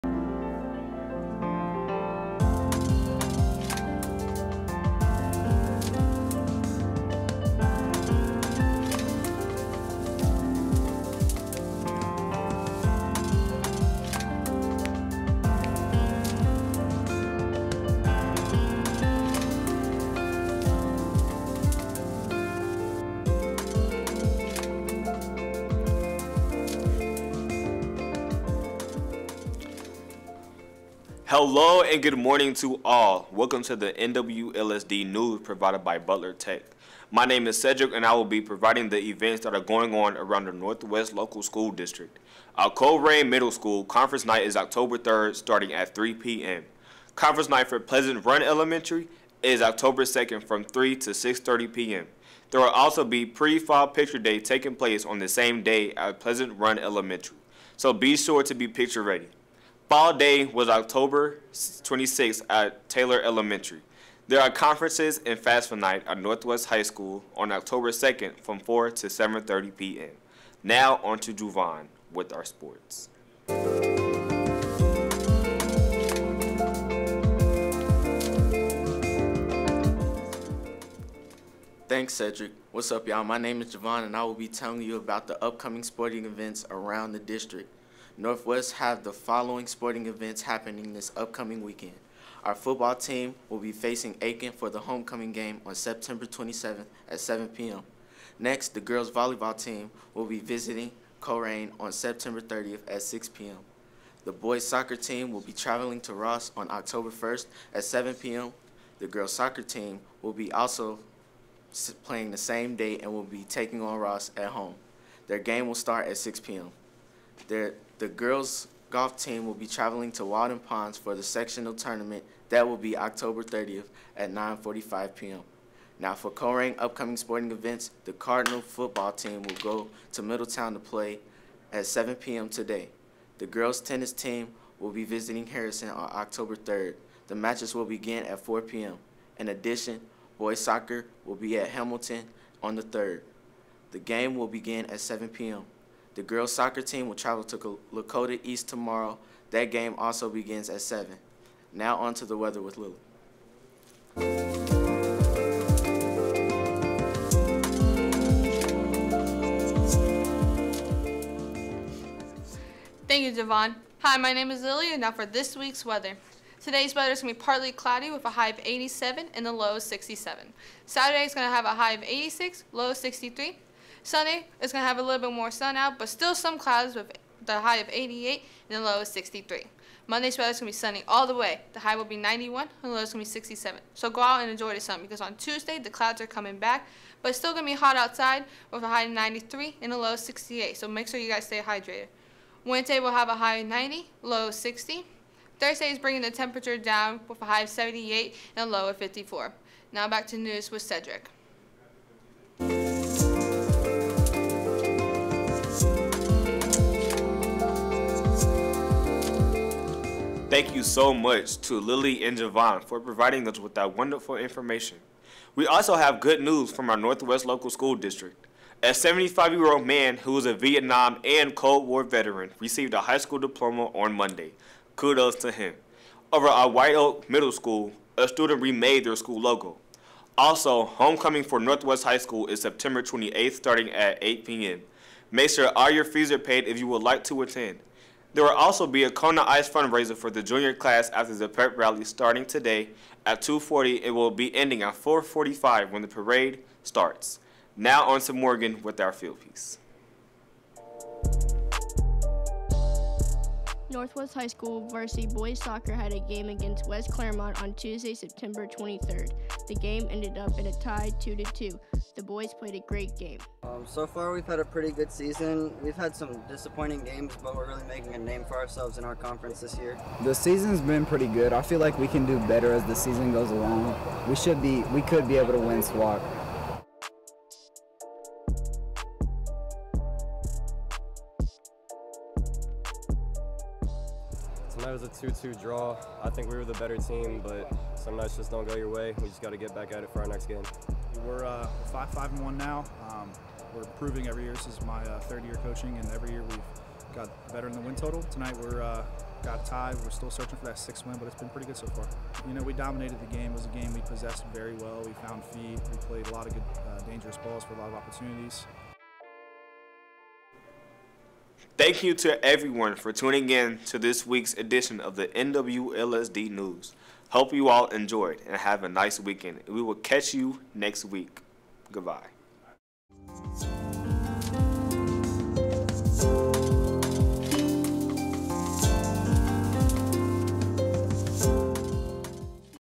NWLSD News - Episode 2
Northwest Local School District News
Covering News, Events, Sports, and Weather.
Field Segment - NWHS Boys Soccer Team
Produced by students in the Butler Tech Digital Media Arts Program at Northwest High School.